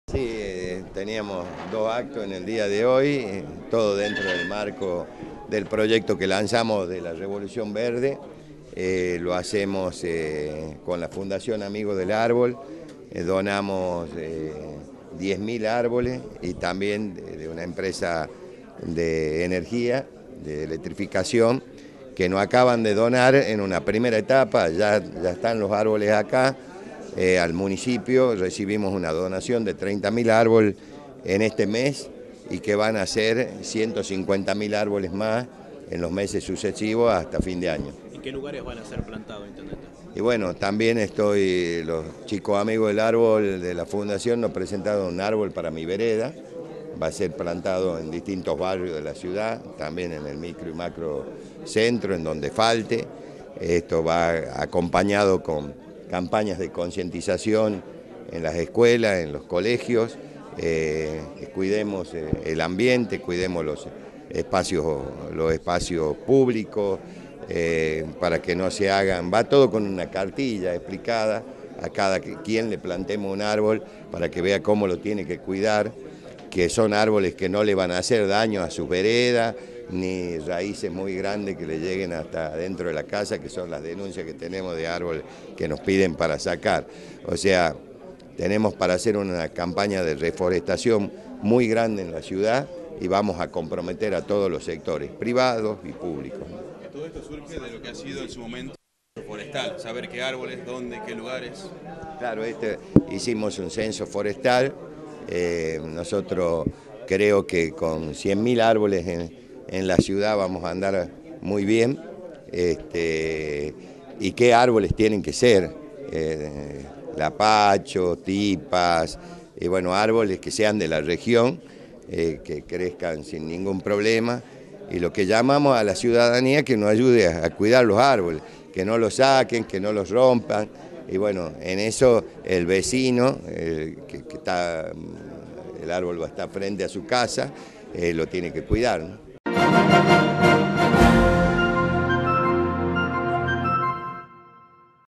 REVOLUCIÓN VERDE, MIGUEL ISA-TV DOS SALTA
Miguel Isa, Intendente de la ciudad de Salta, da detalles sobre la donación de diez mil arboles y el recibimiento de otros treinta mil en el marco de Revolución Verde. (DOS EN LA NOTICIA)